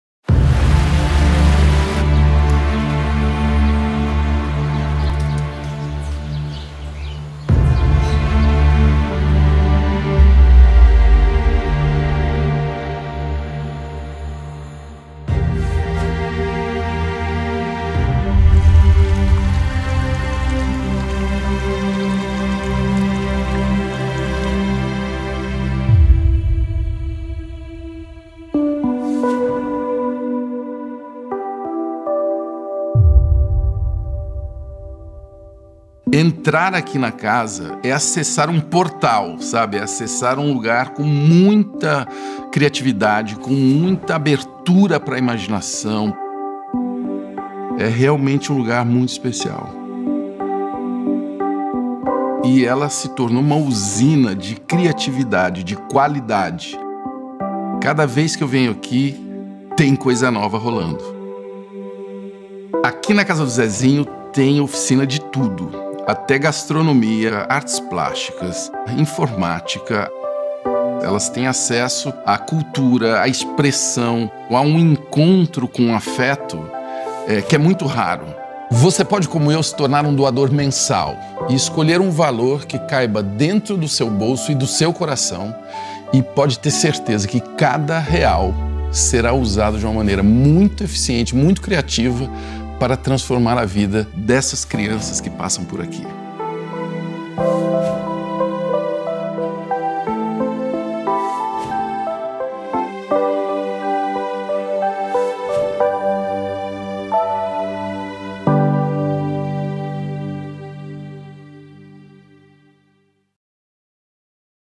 Institucional Casa do Zezinho!
O que pode esperar uma criança ou um jovem que vive em situação de vulnerabilidade social nos bairros da região do Capão Redondo?

Com seu apoio, nossos Zezinhos podem esperar as realizações dos sonhos mais lindos!